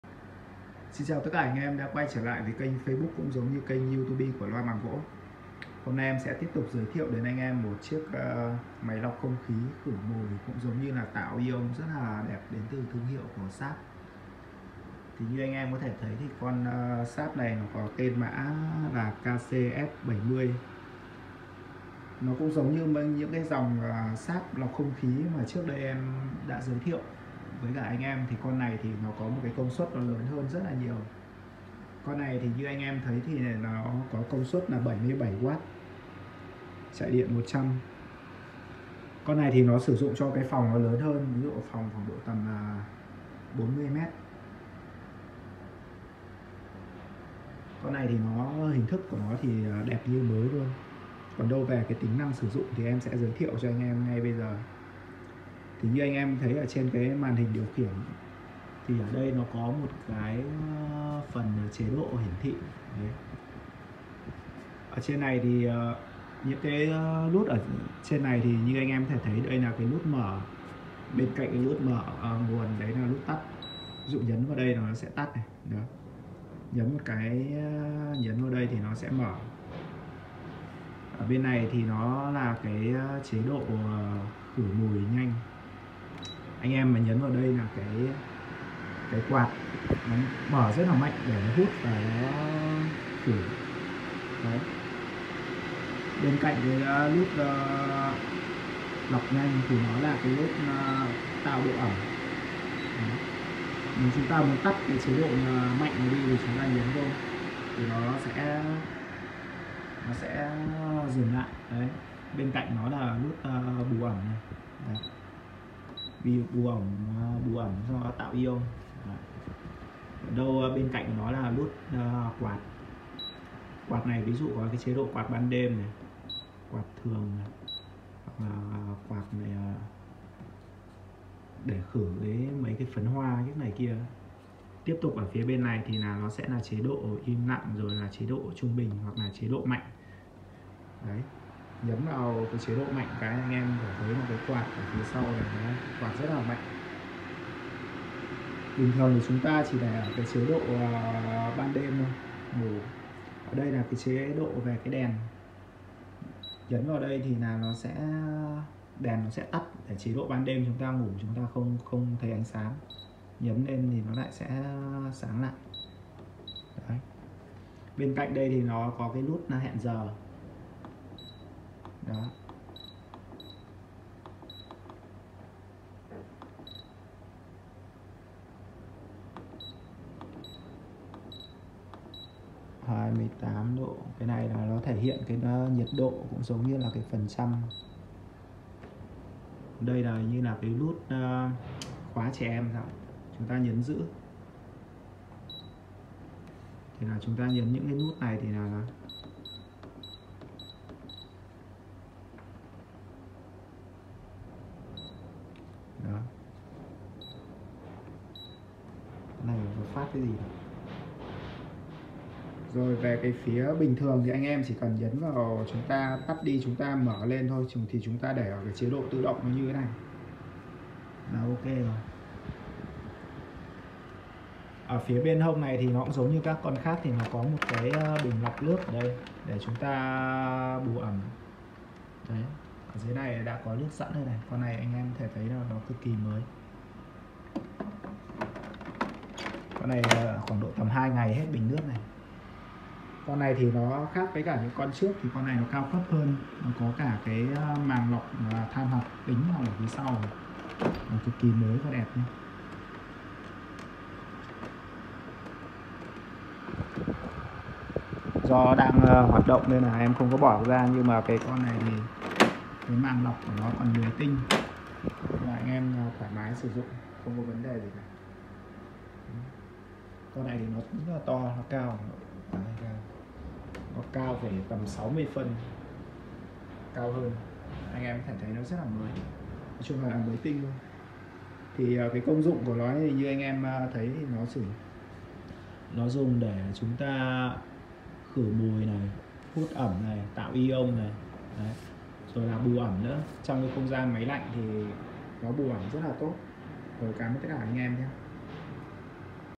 Máy lọc ko khí, khử khuẩn, bù ẩm, tạo Ion Shap KC-F70 hàng VIP Giá 4tr5.
Máy lọc ko khí, khử khuẩn, bù ẩm, tạo Ion Shap KC-F70 hàng VIP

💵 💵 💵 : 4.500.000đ

Máy hoạt động hoàn hảo. 77W Date 2016. Lọc đẹp keng than hoạt tính. Máy xài cho phòng 30-40m2 thoải mái. Sức khỏe là vàng.